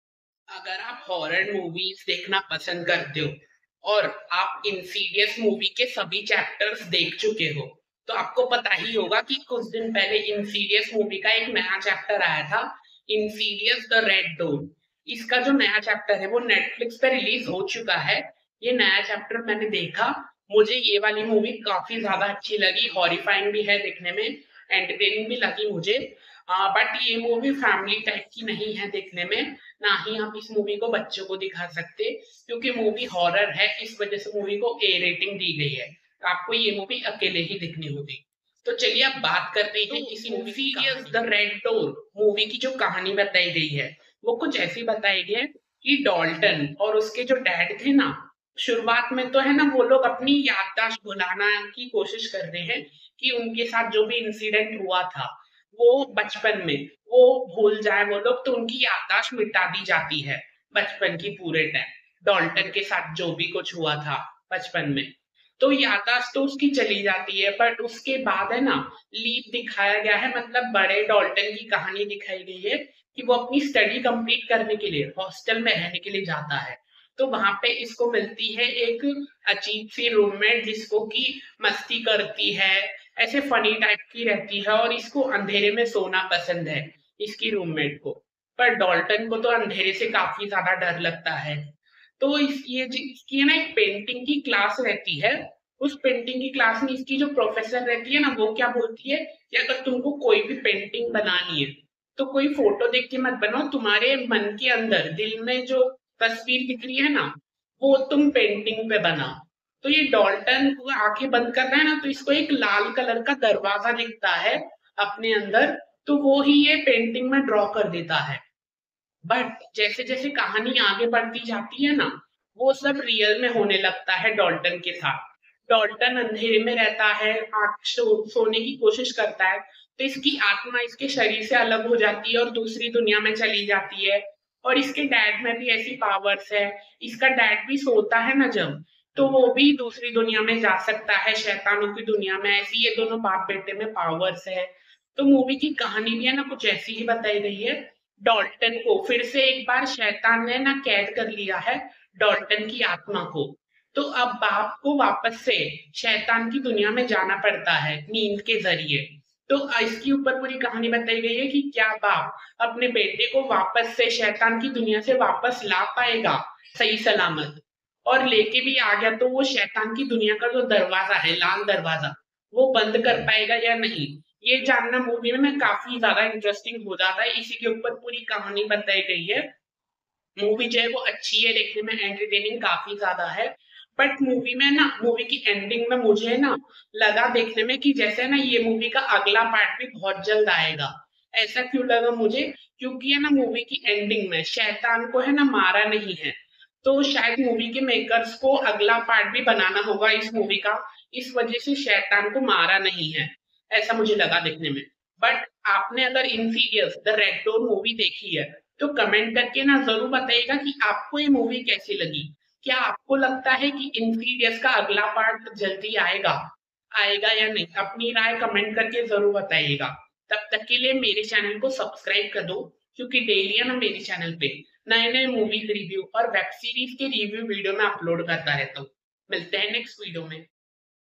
Insidious: The Red Door Movie Review | Netflix New Movie Review
In this video I review Netflix New Movie Insidious: The Red Door Movie Review In Hindi

My Name Is Sahil And I Watch Popular And New Movies And Web Series And Review Them